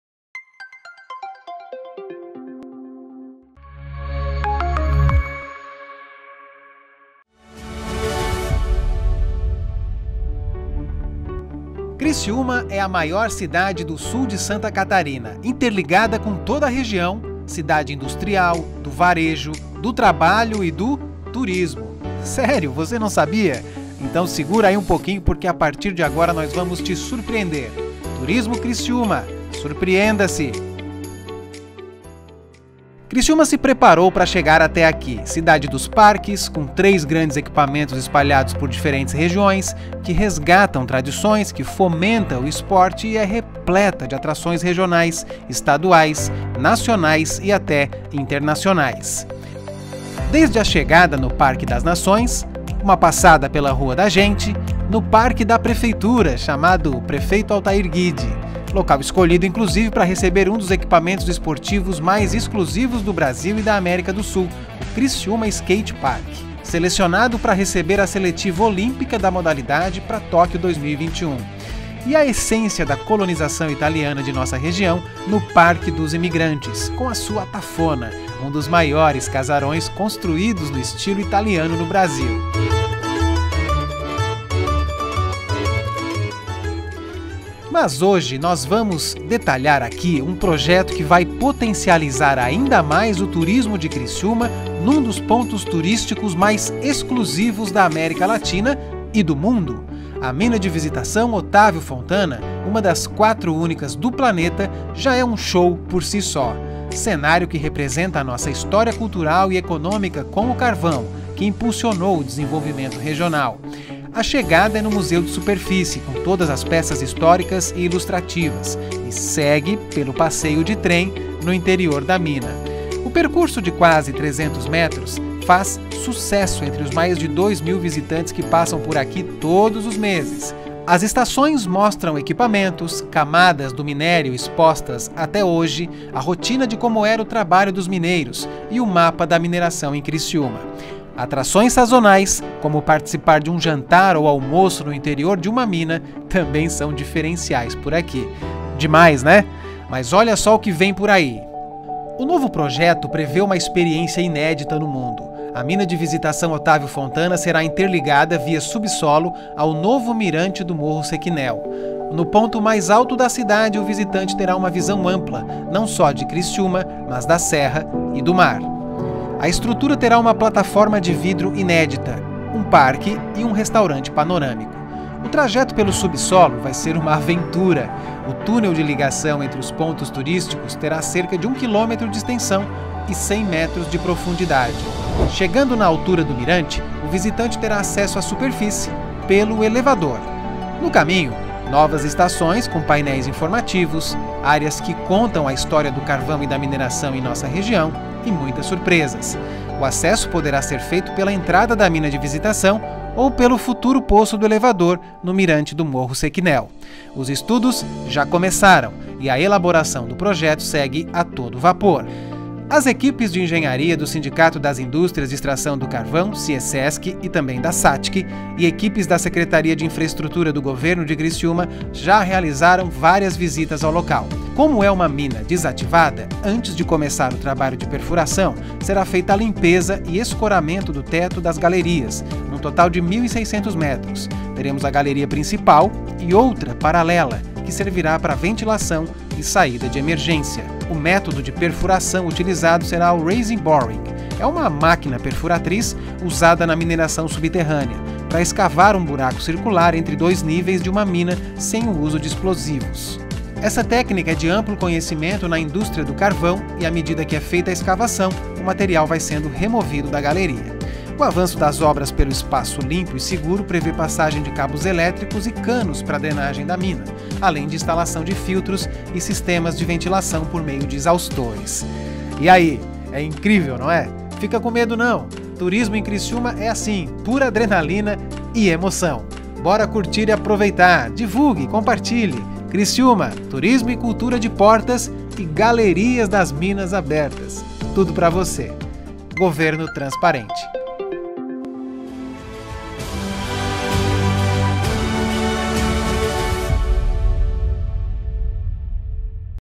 Ligação entre Mina de Visitação e Mirante do Morro Cechinel
A prefeitura de Criciúma lançou o projeto de ligação entre a Mina de Visitação Otávio Fontana e o Mirante Realdo Guglielmi, no Morro Cechinel.